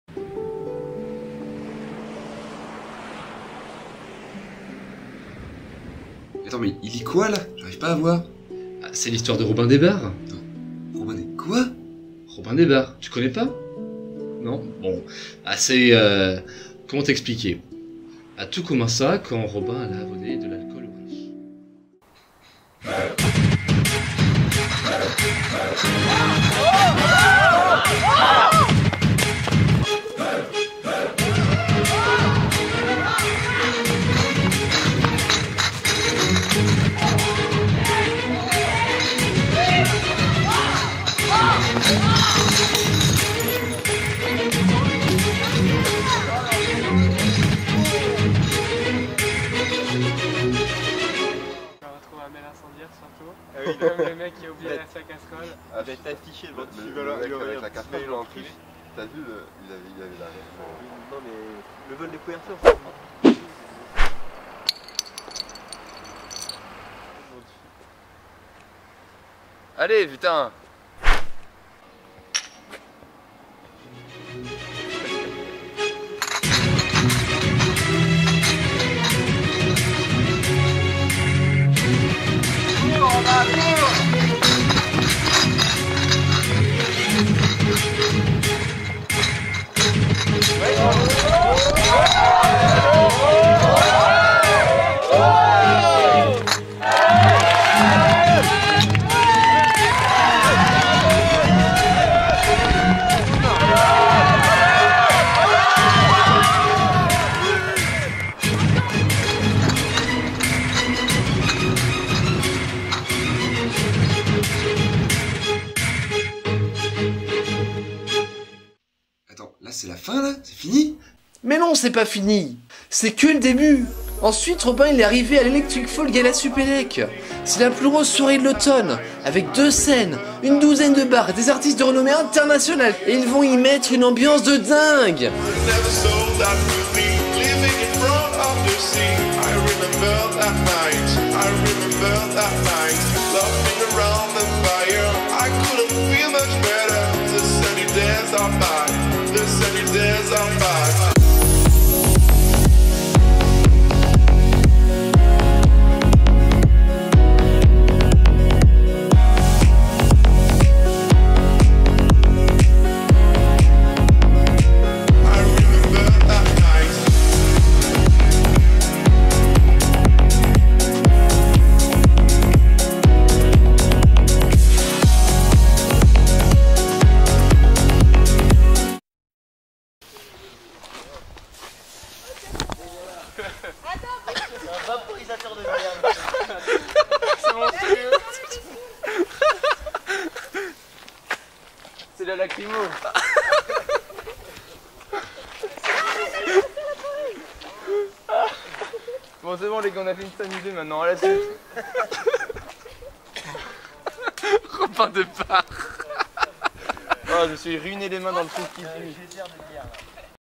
Robin des Bars - Electric Fall Gala Supélec 2015
Quand les plus démunis n'ont plus accès à l'alcool, on peut toujours compter sur Robin des bars !!